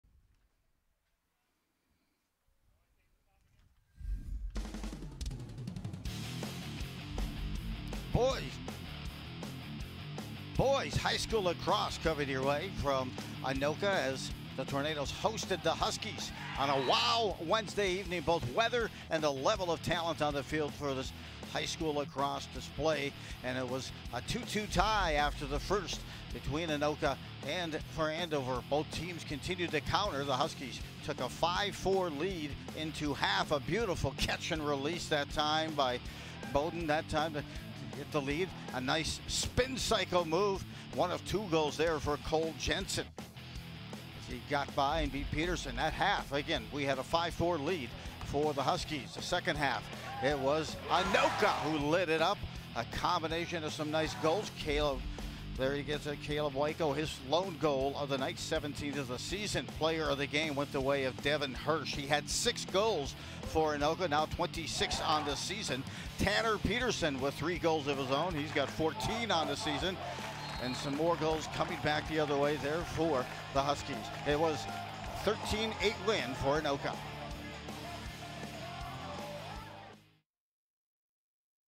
Game Recap: Andover @ Anoka - Boys Lacrosse | Andover High School | QCTV
Anoka takes a 13-8 win over Andover at Goodrich Field!